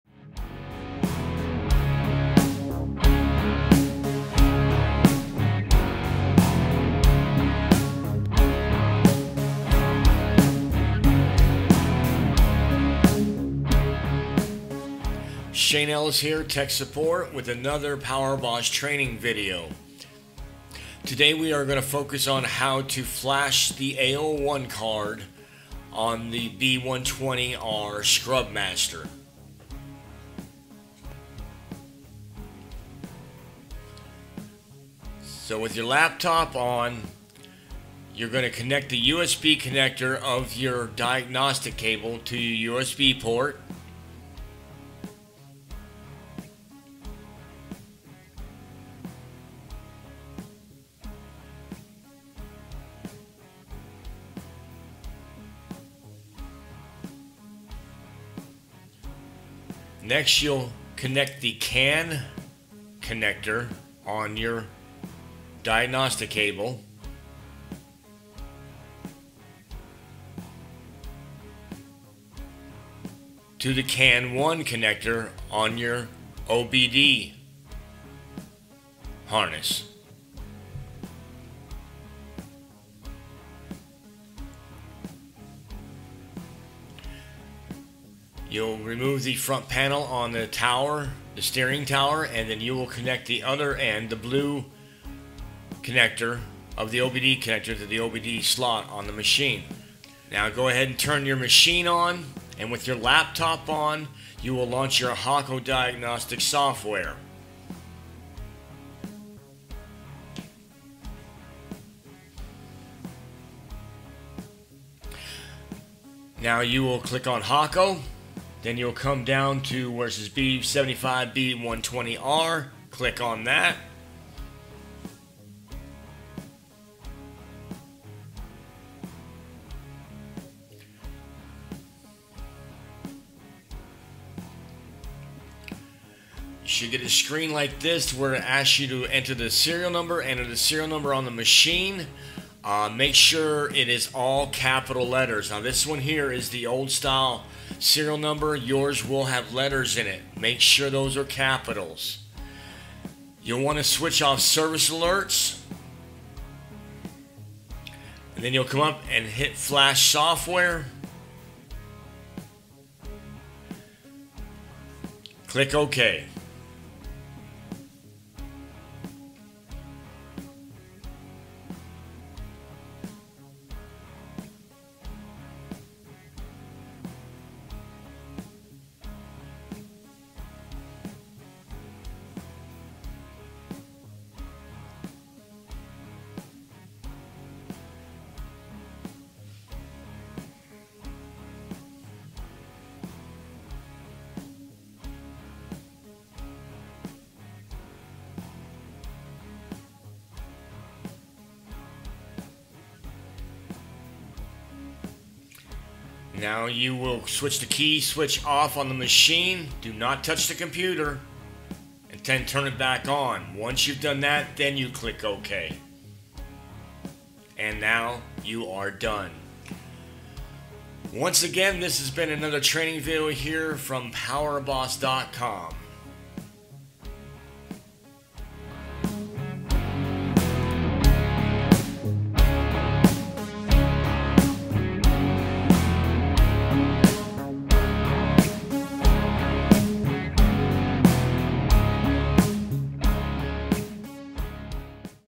Flashing Software B120R
In this video we will explain how to flash the software to the AO1 controller on the Scubmaster B120R